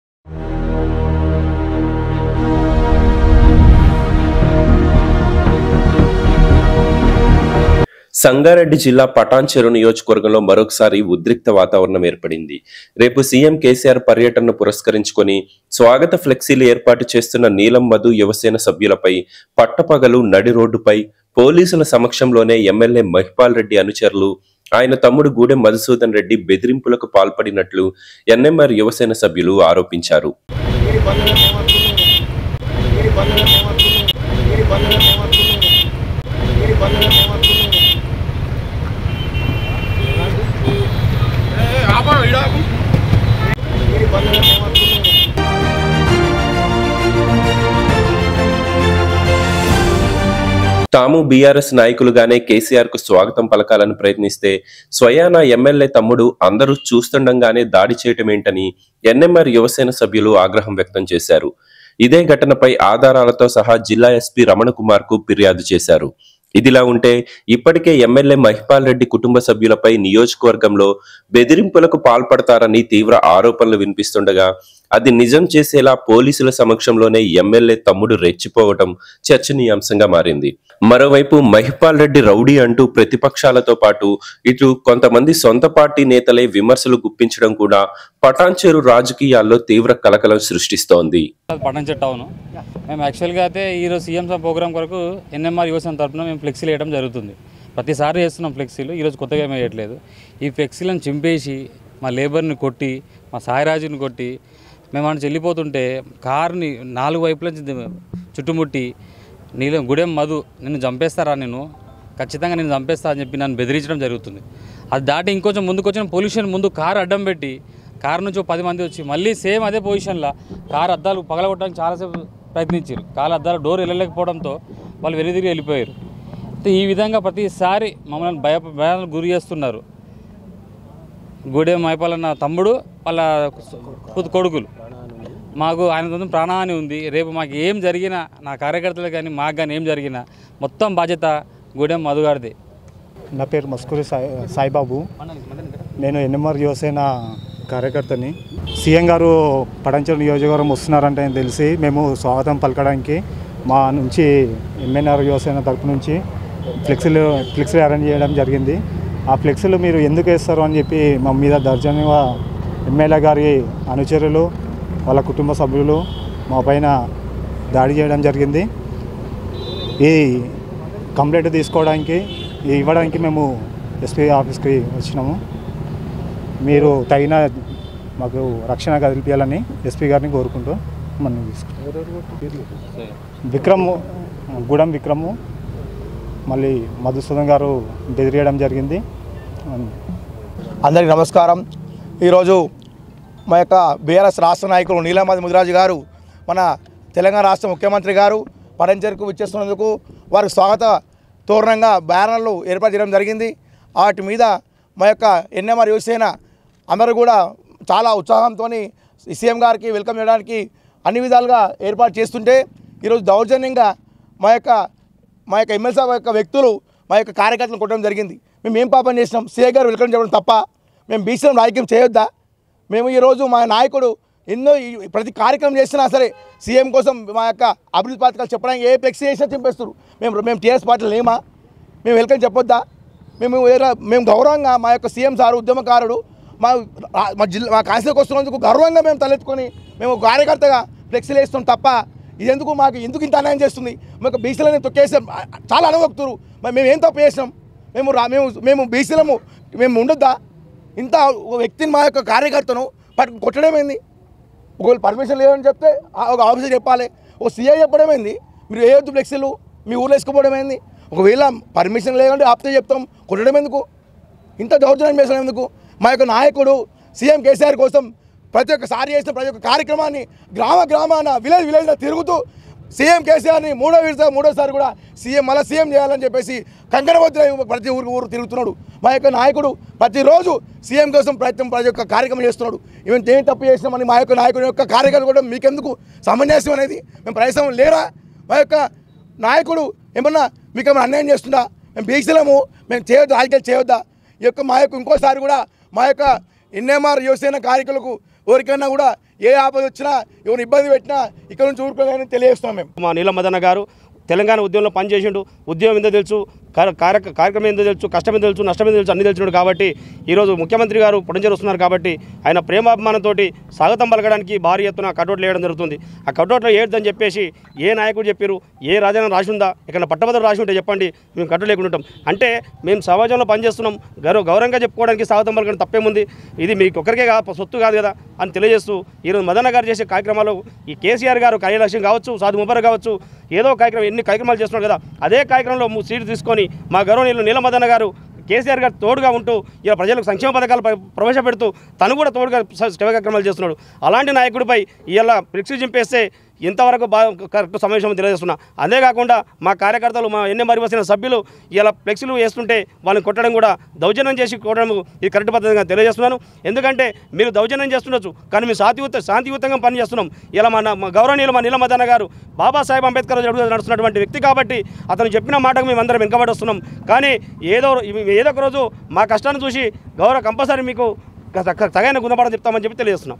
Patancheru : పోలీసుల ముందే నీలం మధు మనుషులకు MLA మహిపాల్ రెడ్డి తమ్ముడి బెదిరింపు | NMR VS GMR
Patancheru : పోలీసుల ముందే నీలం మధు మనుషులకు MLA మహిపాల్ రెడ్డి తమ్ముడి బెదిరింపు | NMR VS GMR | NEWS16 Telugu

Patancheru : ఖబర్ధార్ కొడకా.. నీలం మధు అన్న జోలికొస్తే చెప్పుల దండతో ఊరేగిస్తాం | NMR Yuvasena Fires on Jinnaram Rajesh

 / news16telugu​.  .